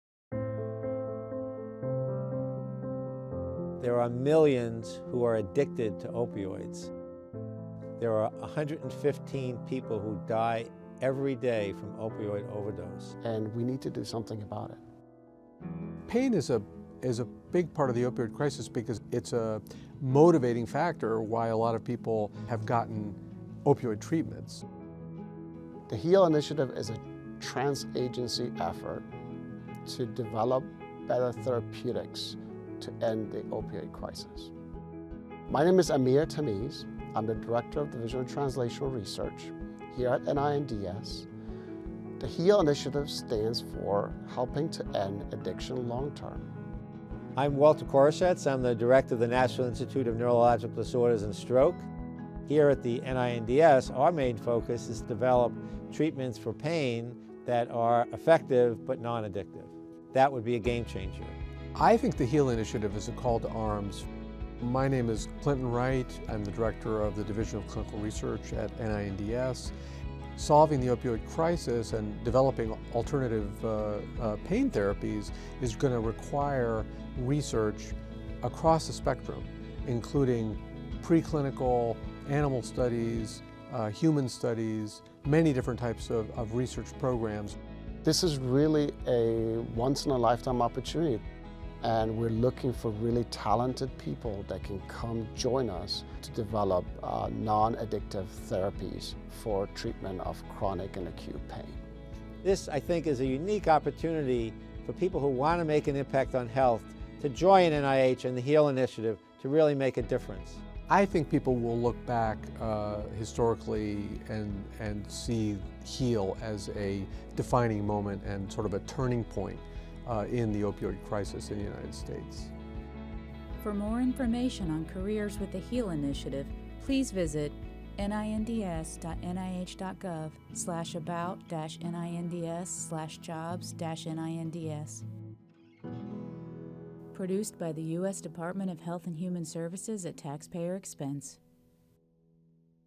Pain and the NIH HEAL Initiative
NIH officials describe NINDS' contribution to the NIH HEAL (Helping to End Addiction Long-term) Initiative, an aggressive, trans-agency effort to speed scientific solutions to stem the national opioid public health crisis.